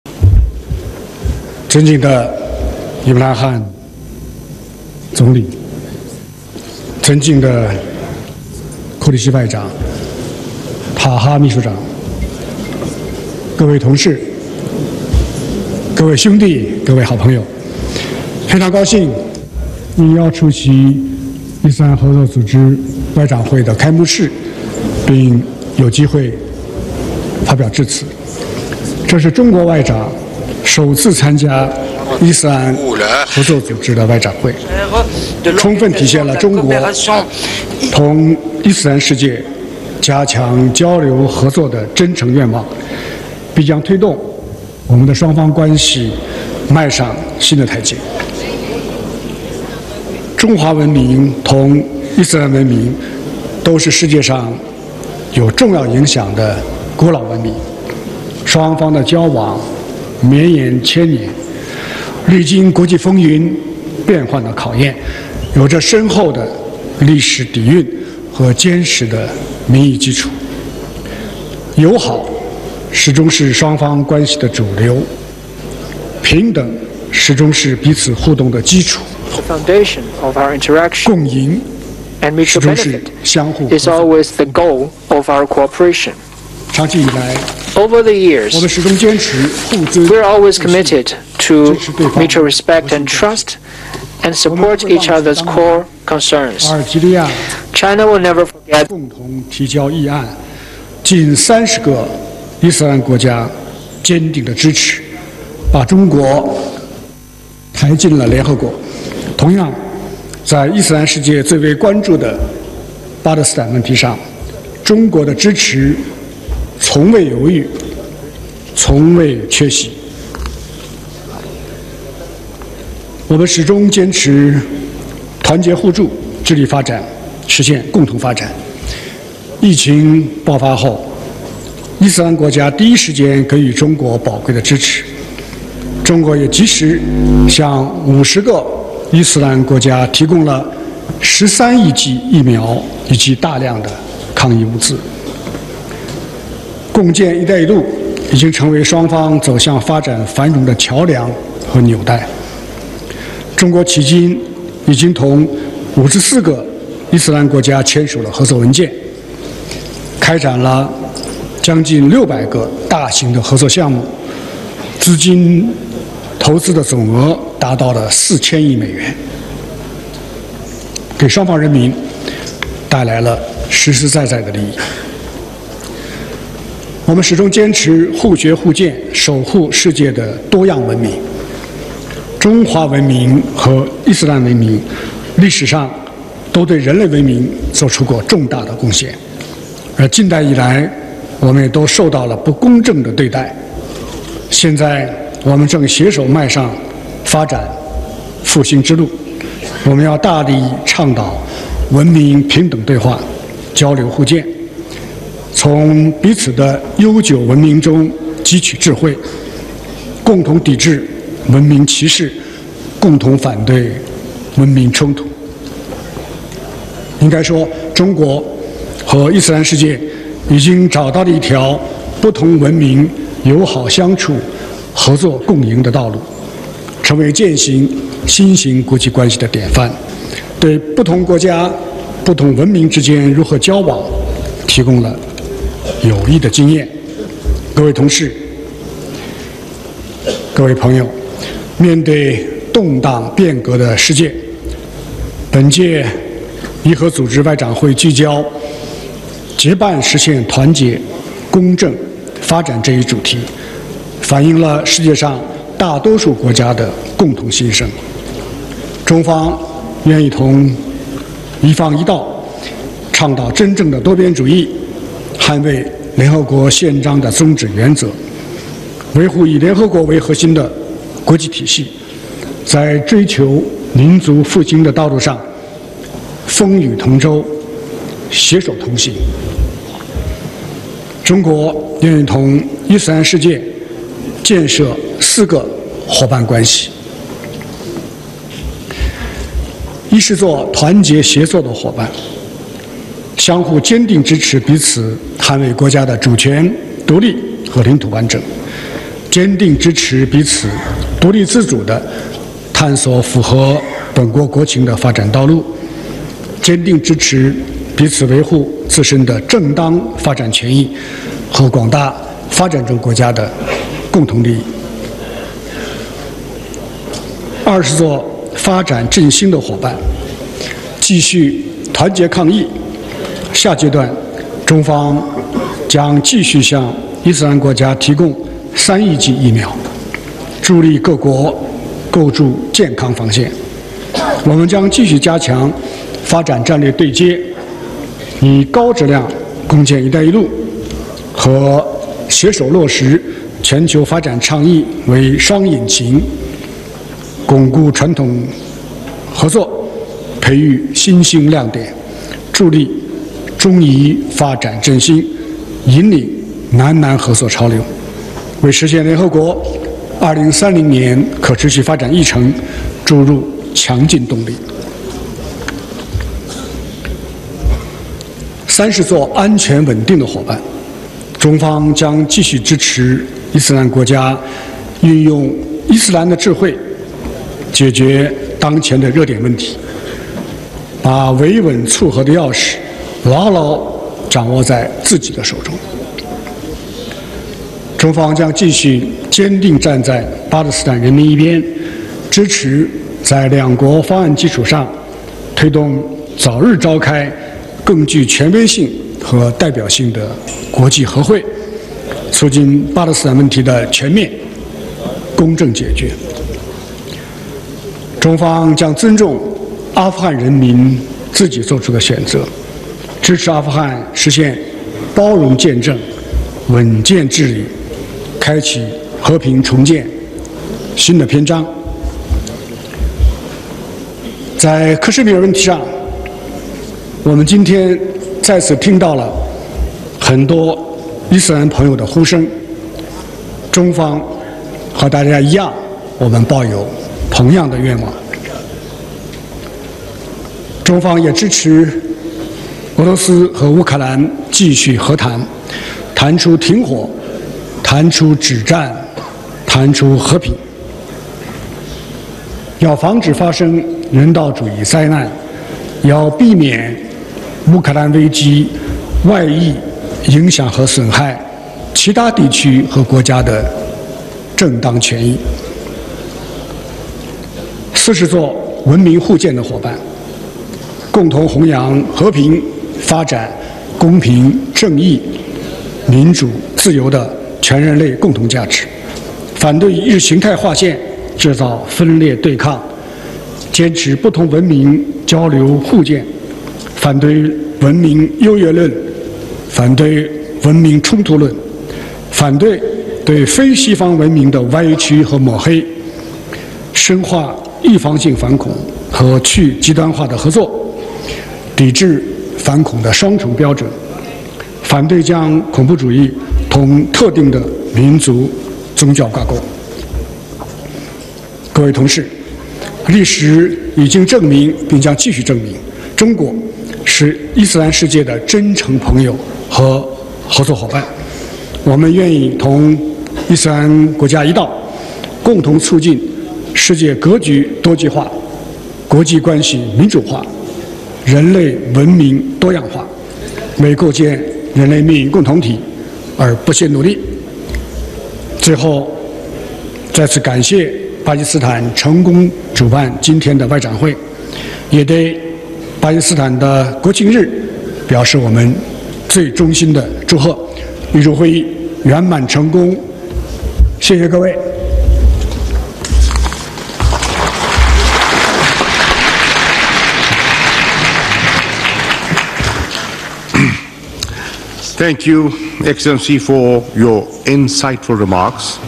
"China to provide 300 million vaccines to Muslim countries " China's Foreign Minister Wang Yi.
China's Foreign Minister Wang Yi
 Speech at 48 OIC Meeting